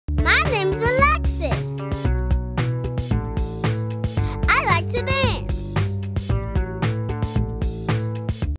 Meet Alexis Fowlkes
This Is Alexis Fowlkes From “Baby Einstein” And “Looney Tunes”.